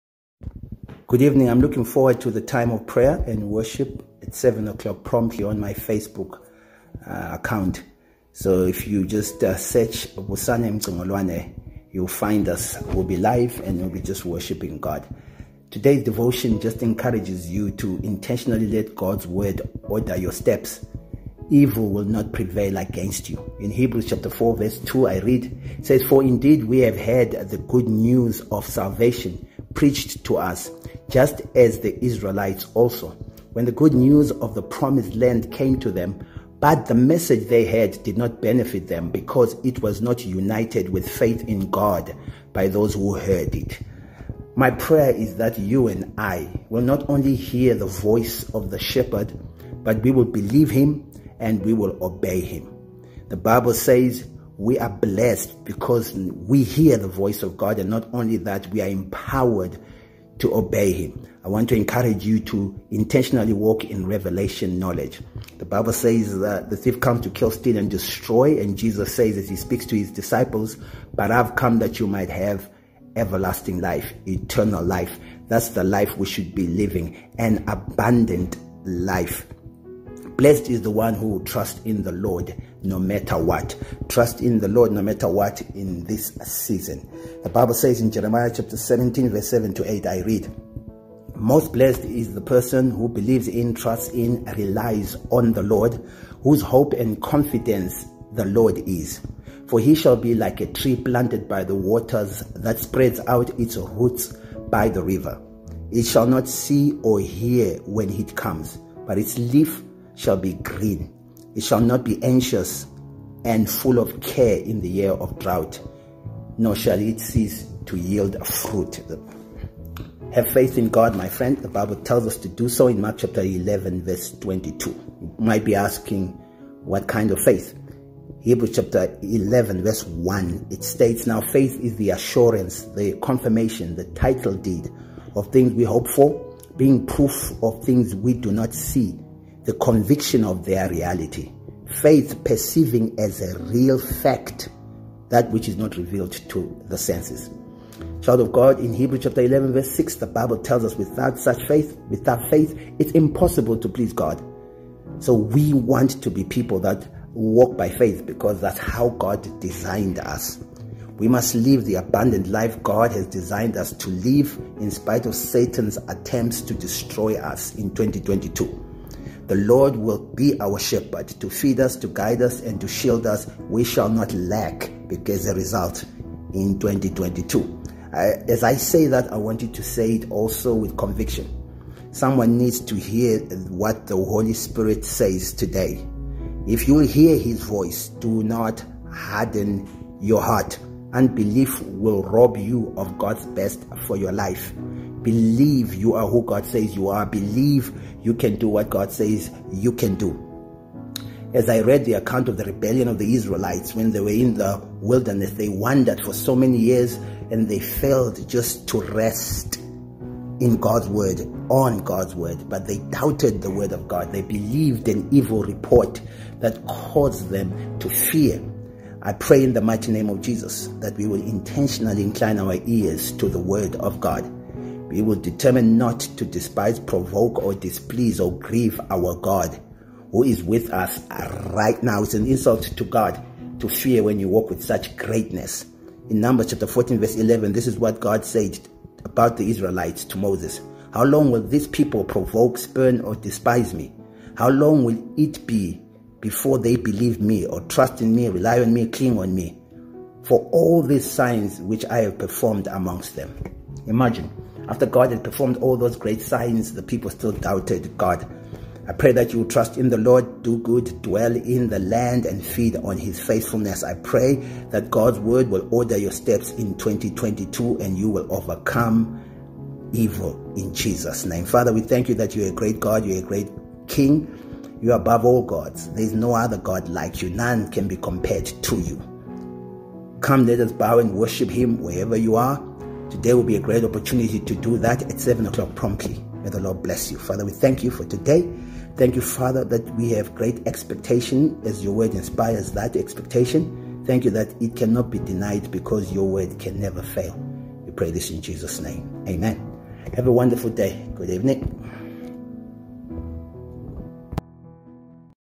GOD'S WORD WILL ORDER OUR STEPS & EVIL WILL NOT PREVAIL AGAINST US
"For indeed we have had the good news [of salvation] preached to us, just as the Israelites also [when the good news of the promised land came to them]; but the message they heard did not benefit them, because it was not united with faith [in God] by those who heard." Hebrews 4:2 AMP

• We are blessed that we hear the voice of our Sovereign Shepherd. He knows us individually & we must follow Him if we are to live the abundant life He designed for us to live (John 10:10, 27).
• Blessed is one who will trust God no matter what.

"[Most] blessed is the man who believes in, trusts in, and relies on the Lord, and whose hope and confidence the Lord is. For he shall be like a tree planted by the waters that spreads out its roots by the river; and it shall not see and fear when heat comes; but its leaf shall be green. It shall not be anxious and full of care in the year of drought, nor shall it cease yielding fruit." Jeremiah 17:7‭-‬8 AMPC

• Have faith in God (Mark 11:22)

"NOW FAITH is the assurance (the confirmation, the title deed) of the things [we] hope for, being the proof of things [we] do not see and the conviction of their reality [faith perceiving as real fact what is not revealed to the senses]...But without faith it is impossible to please and be satisfactory to God. For whoever would come near to God must [necessarily] believe that God exists and that He is the rewarder of those who earnestly and diligently seek Him [out]." Hebrews 11:1‭, ‬6 AMPC.

• We will live the abundant life God has designed for us to live inspite of satan's attempts to steal & destroy us in 2022 (John 10:10).
• THE Lord is our Shepherd [to feed, guide, and shield us],  we shall not lack in 2022 (Psalm 23:1).
• Someone needs to hear what the Holy Spirit says: "Today, if you will hear His voice, Do not harden your hearts."

Unbelief will rob you of God's best for your life. Believe that you are who God says you are & that you can do what He says you can do. Read the account of the rebellion of Israel as they wandered in the wilderness. They did not enter into God's rest because they doubted God's word (Hebrews 3:7‭-‬9‭, ‬11 AMPC). They believed an evil report, perceiving themselves to be grasshoppers & their enemies, giants & concluding it was not possible for them to possess their promise (Numbers 13-14). God was displeased by the Israelites' lack of faith. These are the words He spoke to Moses

"How long will this people provoke (spurn, despise) Me? And how long will it be before they believe Me [trusting in, relying on, clinging to Me], for all the signs which I have performed among them?" Numbers 14:11 AMPC.

Let's determine not to despise, provoke, displease or grieve our God Who is with us right now. He has done so much for us. His presence in our lives means we have access to His wisdom & power, for the Lord gives wisdom; from His mouth comes knowledge and understanding (Proverbs 2:6). Let's determine to incline our ear to His word & believe Him. He will reveal His higher ways and thoughts so we can rest & feed on His faithfulness (Isaiah 55:8-11; Psalm 37:3). We can rest in the assurance that He is able to perform His word even if it seems impossible (Luke 1:37).

• Trust in the LORD, and do good; Dwell in the land, and feed on His faithfulness (Psalm 37:3).
• God's word will order our steps in 2022 and we will not be overcome by evil (Psalm 119:133).

DECLARATION
For the Lord is the great God, the great King above all gods. Come, let us bow down in worship, let us kneel before the Lord our Maker; for He is our God and we are the people of His pasture, the flock under His care. THE Lord is our Shepherd [to feed, guide, and shield us],  we shall not lack in 2022 & nations will see how great our God is.

Inspired by: Psalm 23:1;  95:3, 6‭-‬11